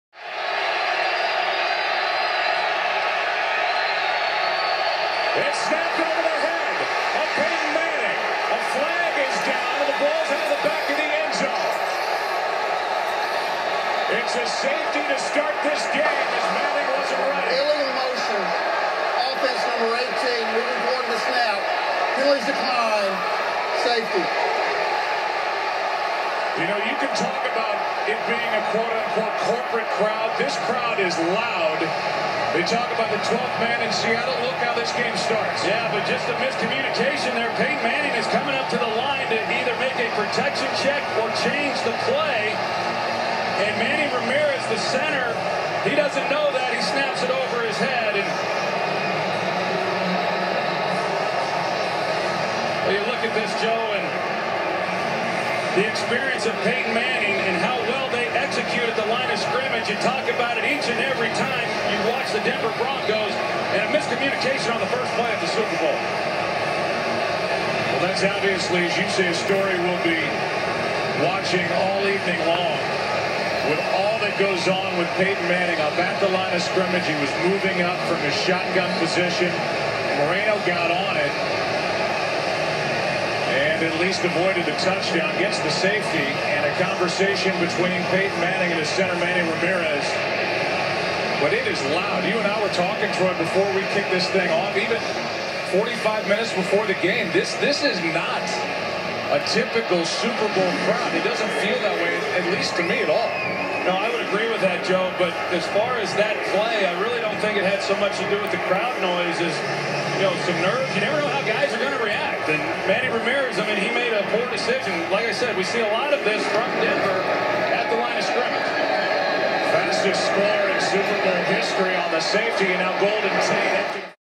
2014 superbowl Opening Safety followed by dustin colquitt PUNT (broncos) punter
ON THE OPENING PLAY OF THE SUPERBOWL, THE SEAHWAKS